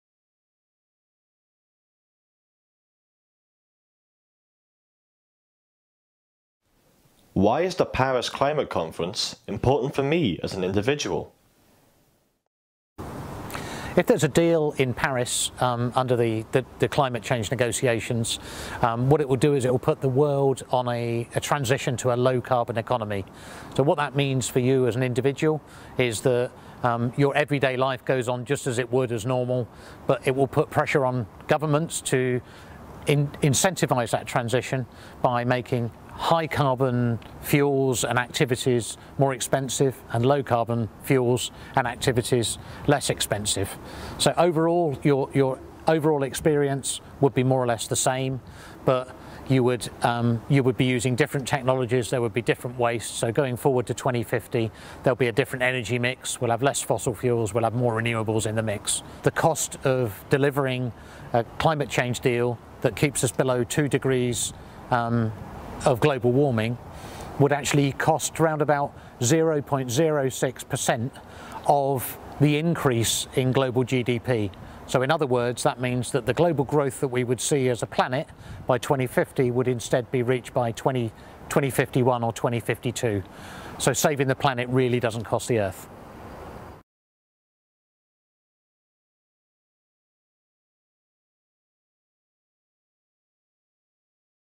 Prof Pete Smith Q1: Why is COP21 important?
Prof. Pete Smith (Aberdeen) answers: Why is the Paris Climate Conference important for me as an individual?